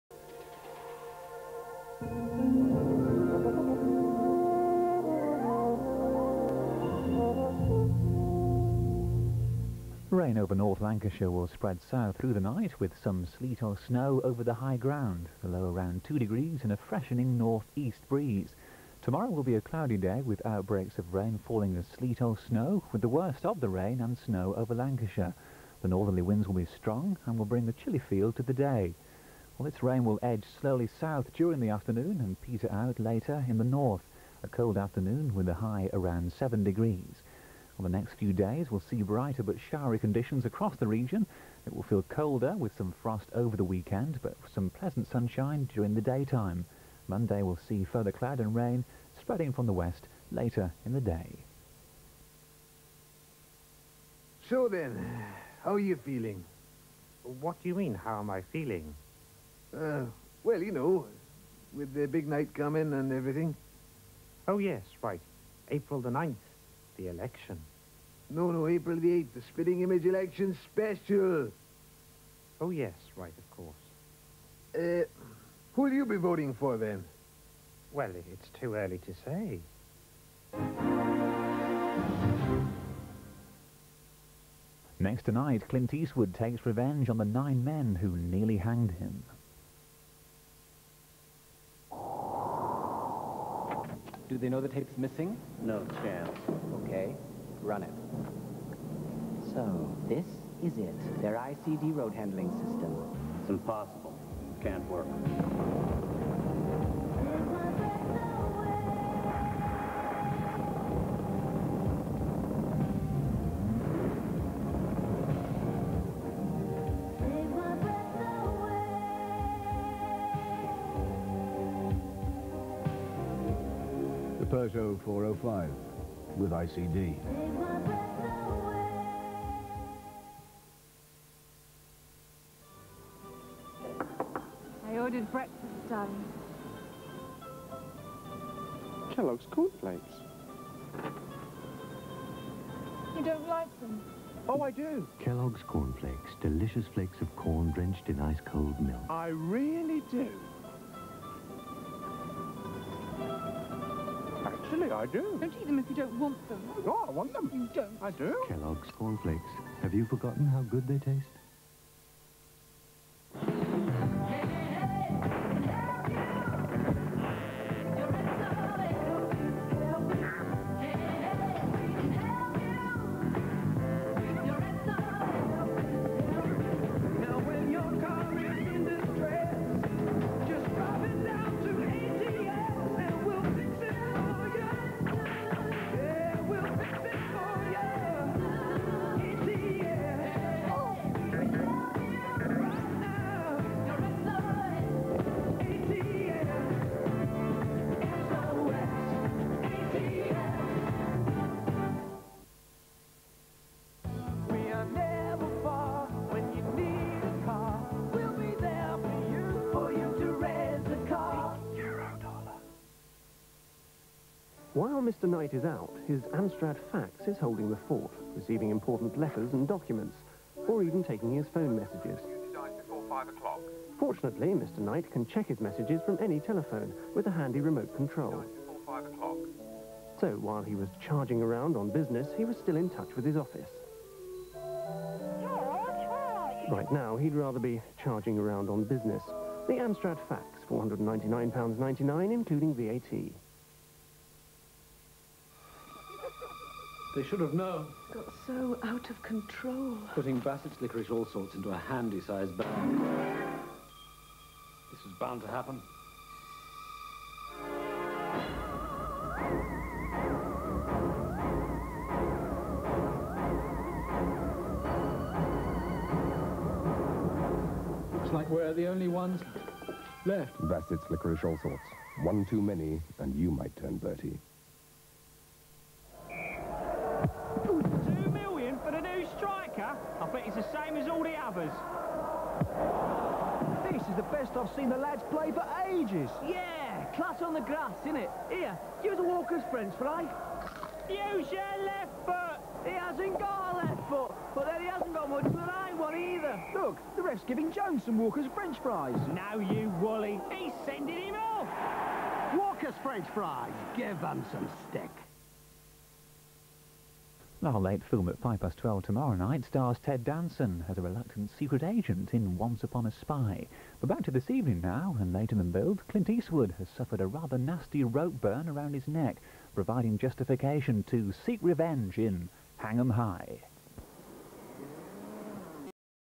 ITV Granada Weather, Adverts and Continuity from April 1992
Granada weather followed by adverts and continuity. Found on a VHS videotape in a charity shop in Blackpool. From April 1992.